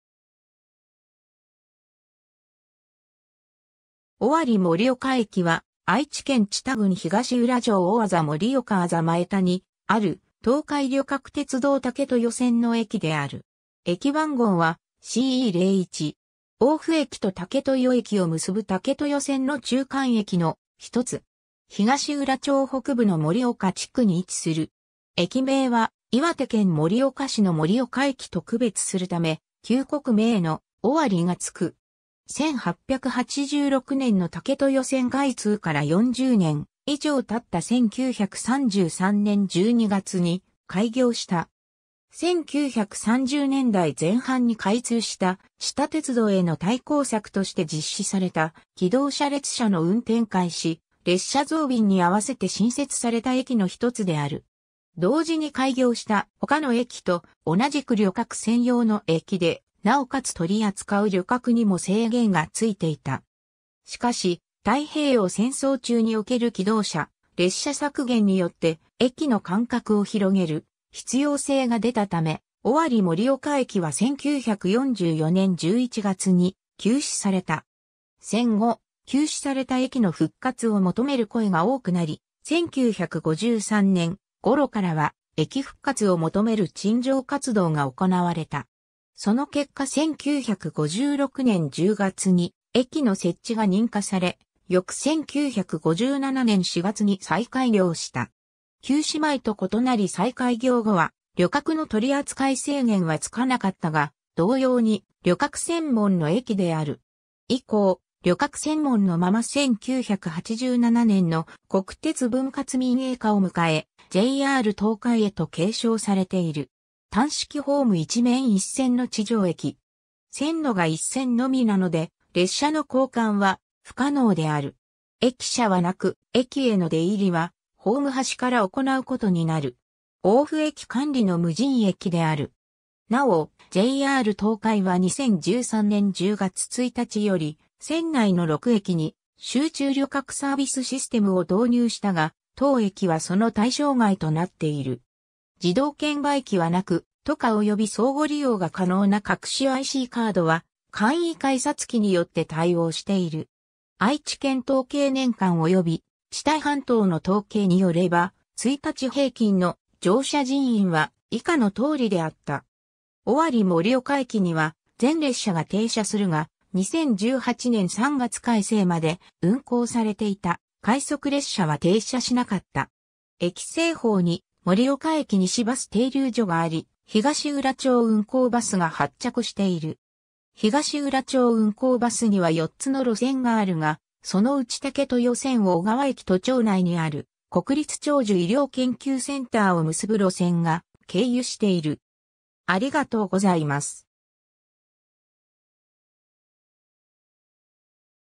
尾張森岡駅
尾張森岡駅（おわりもりおかえき）は、愛知県知多郡東浦町大字森岡字前田にある、東海旅客鉄道（JR東海）武豊線の駅である。
駅番号はCE01。
 大府駅と武豊駅を結ぶ武豊線の中間駅（途中駅）の一つ。
東浦町北部の森岡地区に位置する。
駅名は岩手県盛岡市の盛岡駅と区別するため旧国名の「尾張」が付く。
 1886年（明治19年）の武豊線開通から40年以上経った1933年（昭和8年）12月に開業した。
1930年代前半に開通した知多鉄道（現・名鉄河和線）への対抗策として実施された気動車列車の運転開始・列車増便（1933年8月実施）にあわせて新設された駅の一つである。
同時に開業した他の駅と同じく旅客専用の駅で、なおかつ取り扱う旅客にも制限が付いていた。
しかし太平洋戦争中における気動車列車削減によって駅の間隔を広げる必要性が出たため、尾張森岡駅は1944年（昭和19年）11月に休止された。
 戦後、休止された駅の復活を求める声が多くなり、1953年ごろからは駅復活を求める陳情活動が行われた。
その結果1956年（昭和31年）10月に駅の設置が認可され、翌1957年（昭和32年）4月に再開業した。
休止前と異なり再開業後は旅客の取り扱い制限は付かなかったが、同様に旅客専門の駅である。
以降、旅客専門のまま1987年（昭和62年）の国鉄分割民営化を迎え、JR東海へと継承されている。
 単式ホーム1面1線の地上駅。
線路が1線のみなので、列車の交換は不可能である。
駅舎はなく、駅への出入りはホーム端（踏切横）から行うことになる。
 大府駅管理の無人駅（駅員無配置駅）である。
なお、JR東海は2013年10月1日より線内の6駅に「集中旅客サービスシステム」を導入したが、当駅はその対象外となっている。
自動券売機はなく、TOICAおよび相互利用が可能な各種ICカードは簡易改札機によって対応している。
 「愛知県統計年鑑」および「知多半島の統計」によれば、1日平均の乗車人員は以下の通りであった。
 尾張森岡駅には、全列車が停車するが、2018年3月改正まで運行されていた、快速列車は停車しなかった。
 駅西方に「森岡駅西」バス停留所（バス停）があり、東浦町運行バス（う・ら・ら）が発着している。
東浦町運行バスには4つの路線があるが、そのうち武豊線緒川駅と町内にある国立長寿医療研究センターを結ぶ路線が経由している。